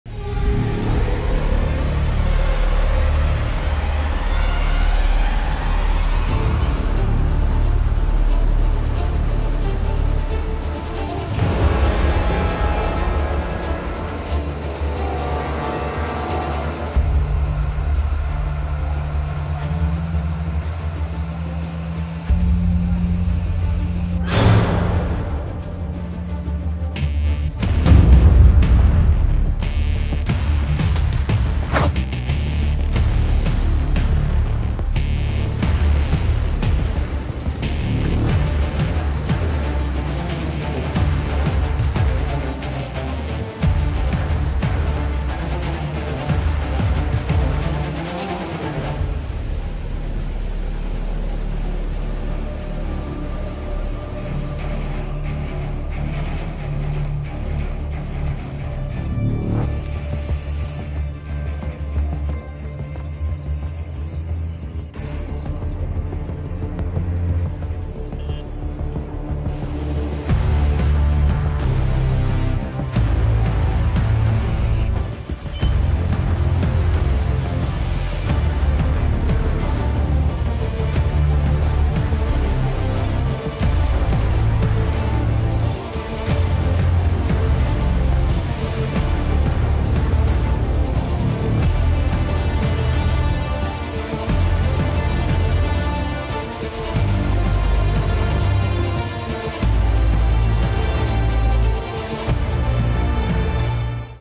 นาทีน้ำป่า! ถล่มขุนยวม พัดบ้านเสียหาย | PPTV Online
เปิดนาทีน้ำป่า! ถล่มขุนยวม แม่ฮ่องสอน พัดบ้านเรือนเสียหาย ถนนบางสายถูกตัดขาด